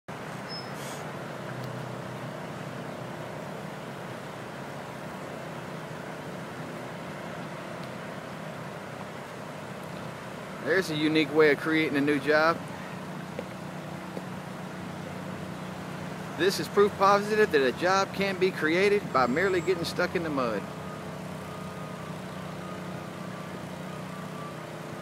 Truck stuck in mud epic fail ! Highway worker gets newly created job
Truck gets stuck in the mud and a disgruntled highway worker has to dig it out!  Jobs being created right here in Baton Rouge Louisiana! TIP US HERE; we lost everything in the flood in Louisiana.  Help us raise money to rebuild share the link please